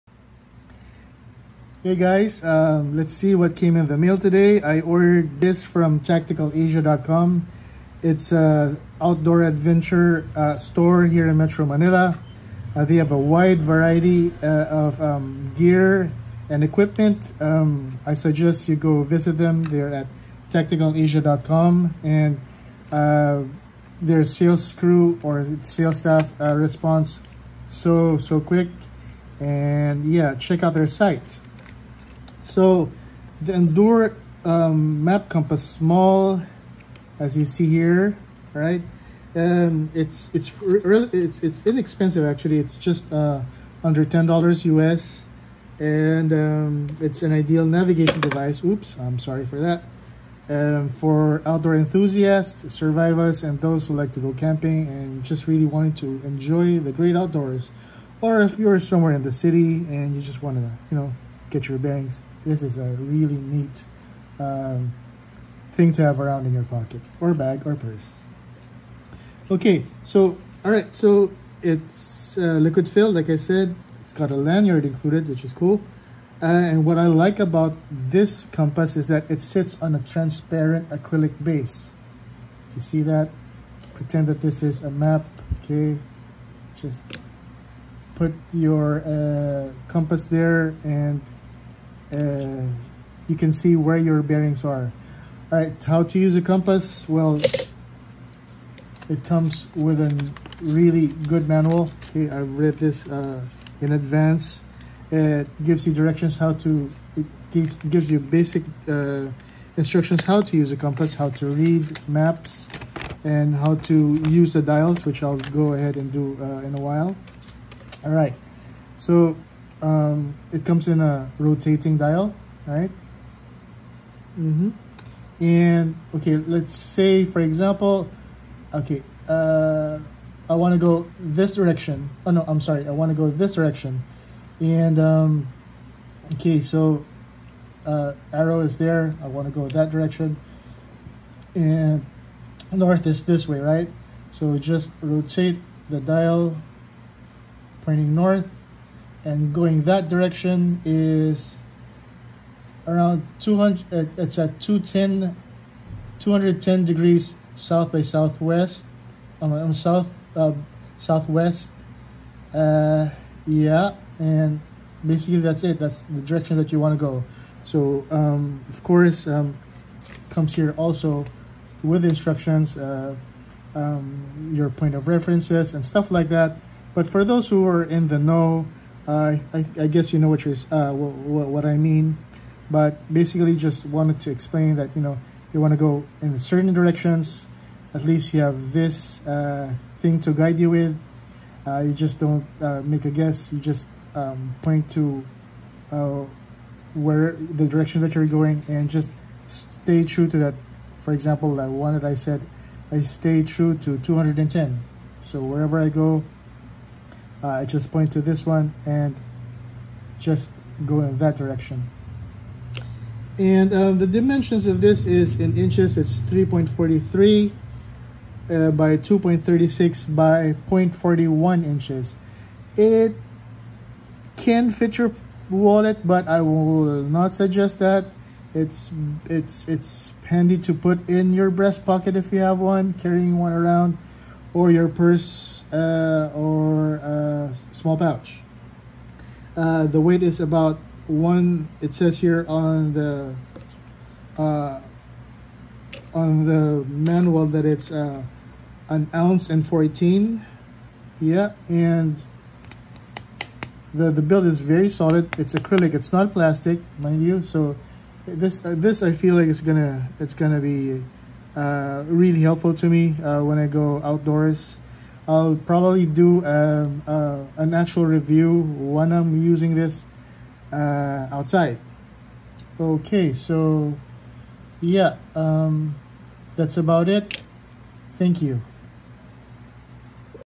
NDuR Map Compass Small Review (2022) -Great Adventure Outdoors
EDC Collection featuring the unfancy but nevertheless cool NDuR Map Compass Small. Simple, small, and fun to use.  The NDuR compass adds a new thrill to your outdoor activity. Taking it out from your pocket, even in the city, wont make you look odd. Forge ahead with the NDuR Map Compass Small!
BIG THANKS TO TACTICALASIA.COM